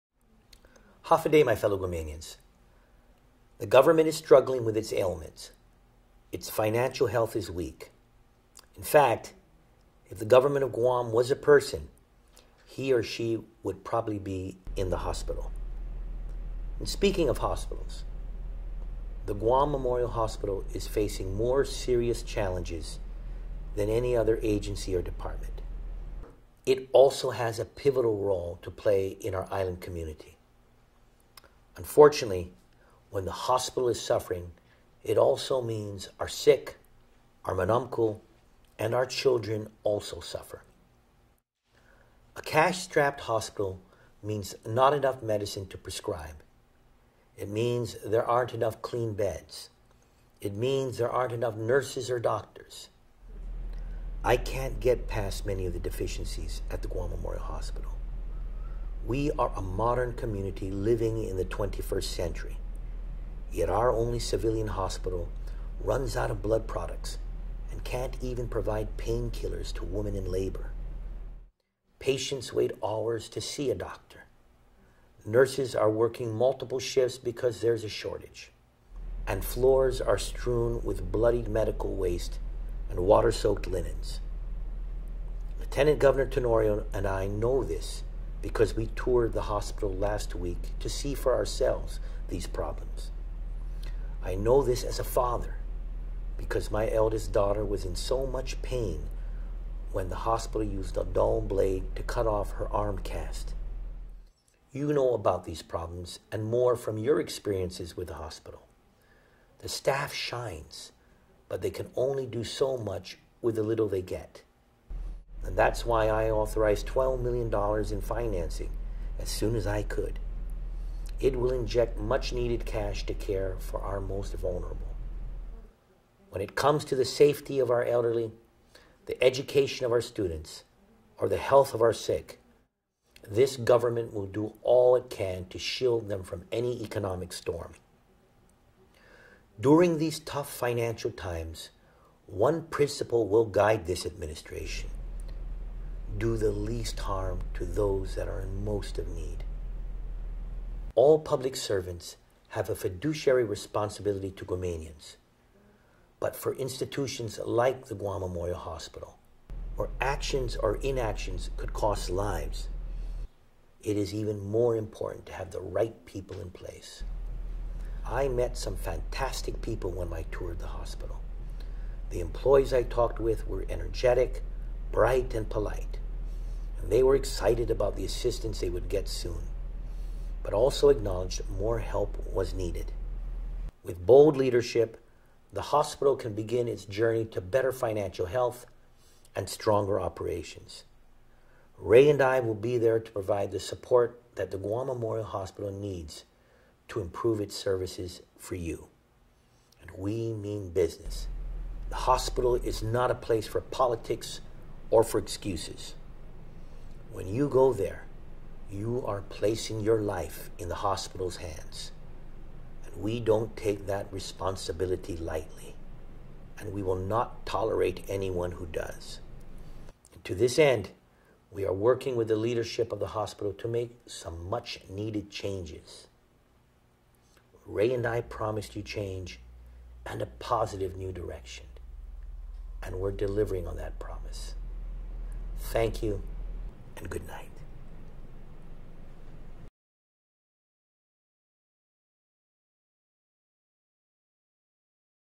Governor Calvo's Weekly Radio Address - February 7
The government is struggling with its ailments. Its financial health is weak. In fact, if the Government of Guam was a person, he or she would probably be in the hospital.